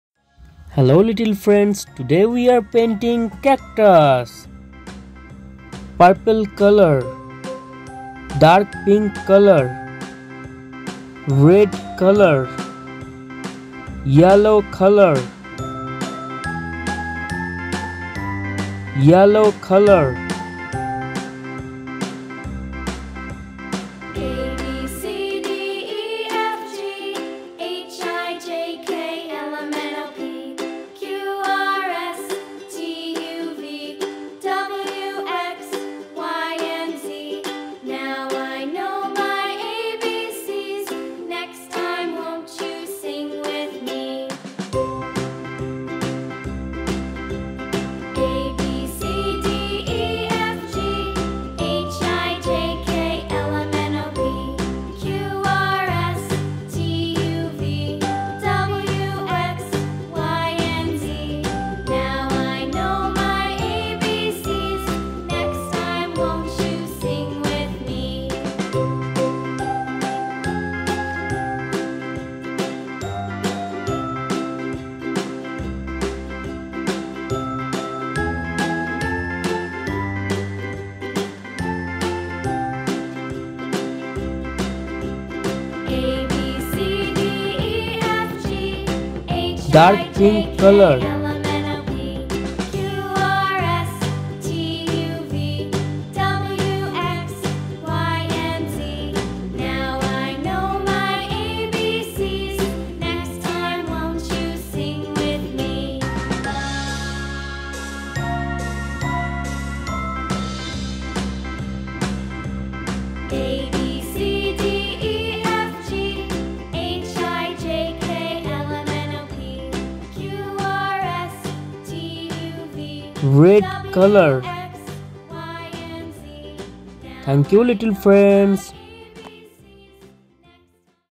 Cute Cactus 🌵 Drawing, Painting and Colouring for kids and toddlers| abcd rhymes song for kids #88
Cute Cactus 🌵 Drawing, Painting and Colouring for kids and toddlers| abcd rhymes song for kids